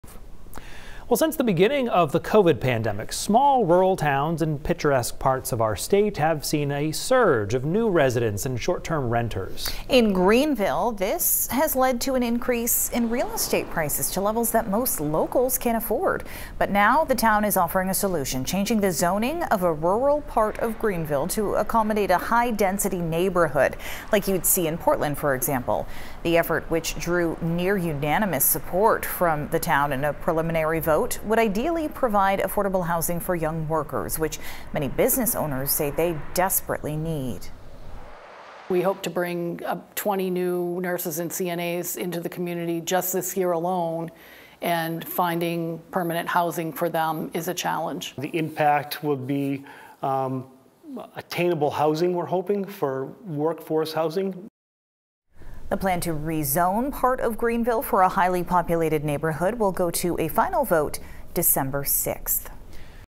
Greenville offers plan to make housing more affordable
The town is offering an affordable housing solution to change zoning of a rural area to accommodate a high-density neighborhood.